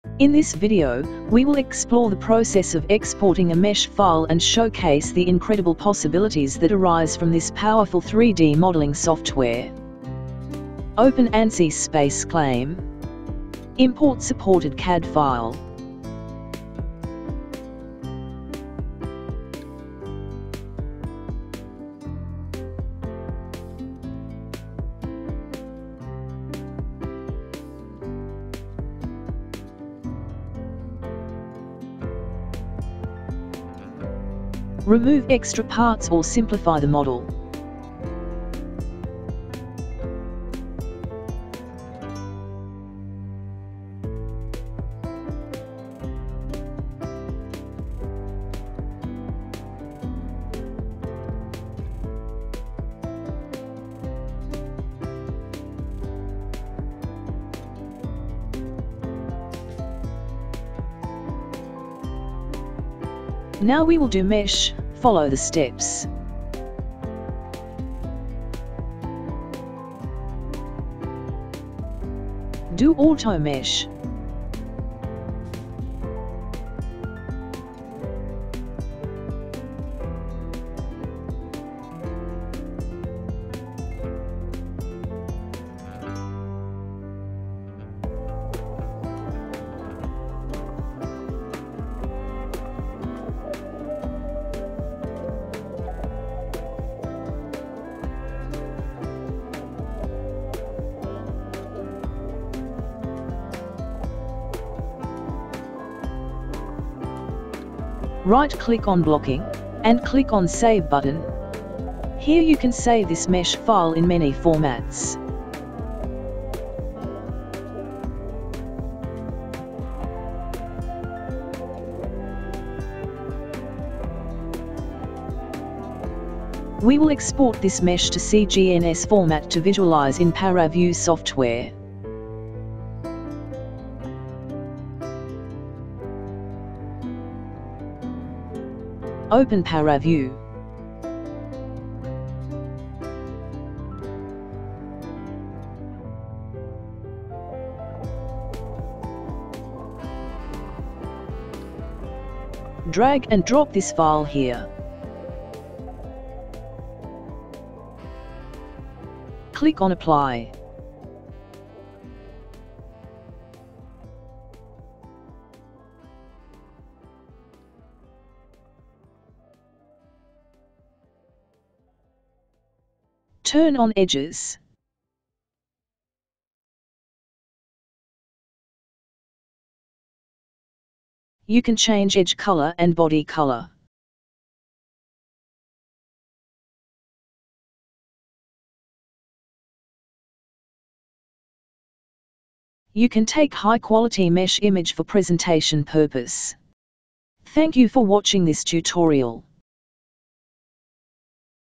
✅Mesh Mastery: Exploring Ansys SpaceClaim's Exporting and Transforming Capabilities 💡
Step into the world of mesh mastery with Ansys SpaceClaim! In this captivating video, witness the transformative capabilities of Ansys SpaceClaim as we delve into the process of exporting mesh files. 🚀🎨🔧✨